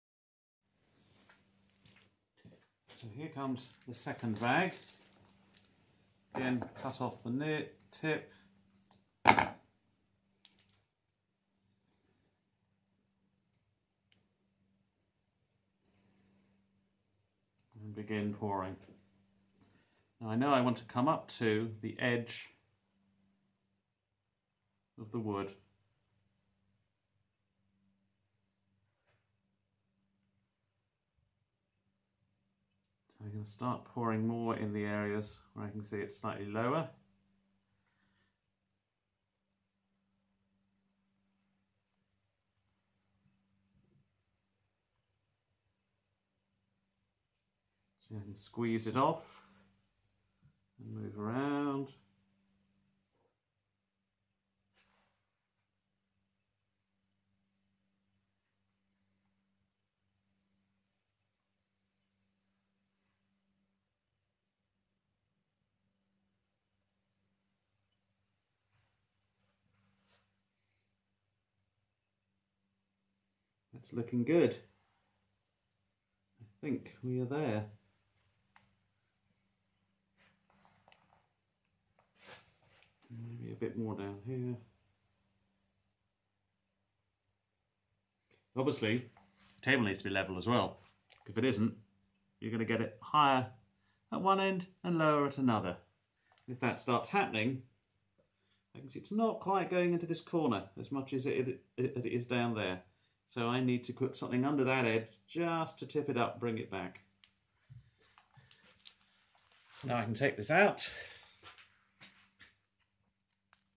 How to Make a Dalle de Verre (Version 1) - Part 7
Adding the final amount of resin around the Dalles.